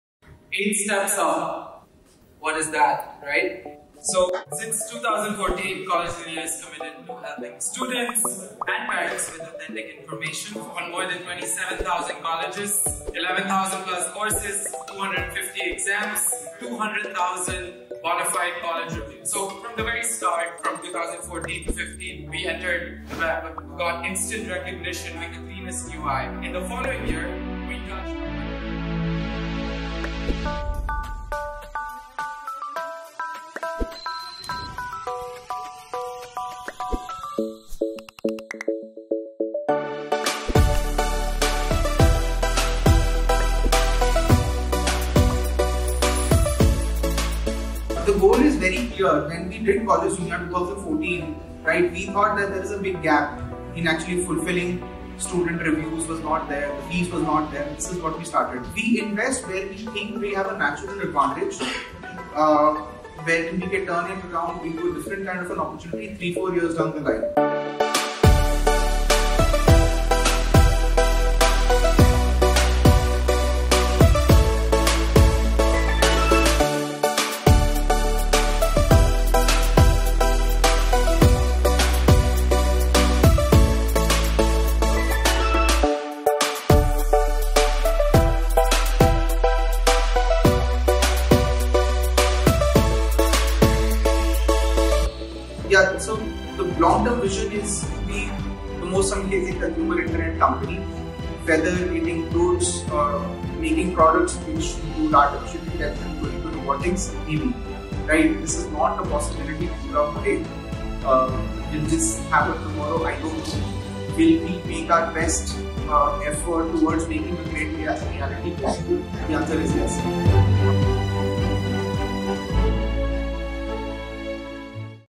8 Steps Up!
These past 8 years have been exceptional and thrilling, so the celebration also had to be a little special.

But, what's so special about us?

The humans at work. The people who have been working here since the beginning, and also the ones who joined us or parted ways. This year we celebrate our 8 years of existence, our glorious achievements, and the people who have worked so hard to make it possible.

In these 8 years, we have stepped up our game and now with a family of over 1000+ employees, we are looking forward to stepping up our game even further.

Here's to coming closer to a decade of challenges & hard work.
Cheers!!